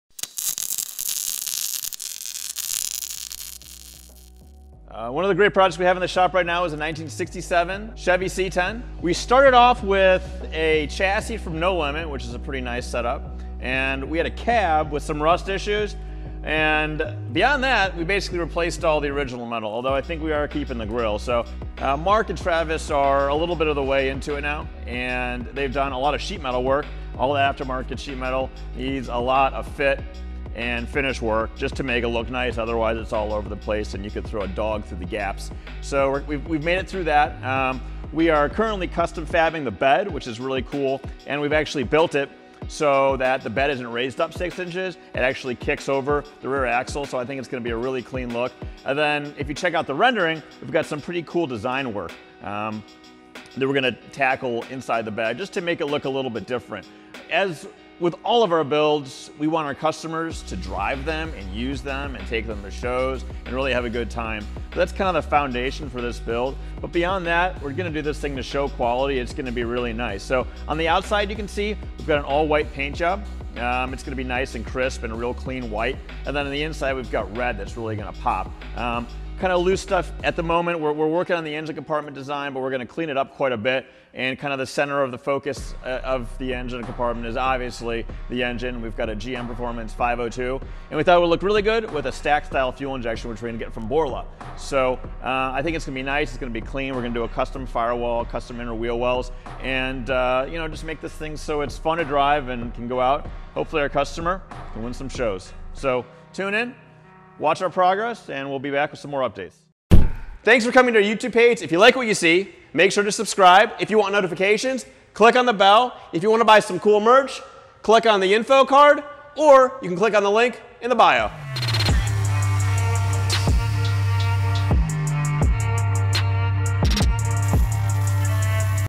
1967 Chevy C10 Build Introduction
In this video, Noah goes over the plan of a new build in the shop, a 67 C10.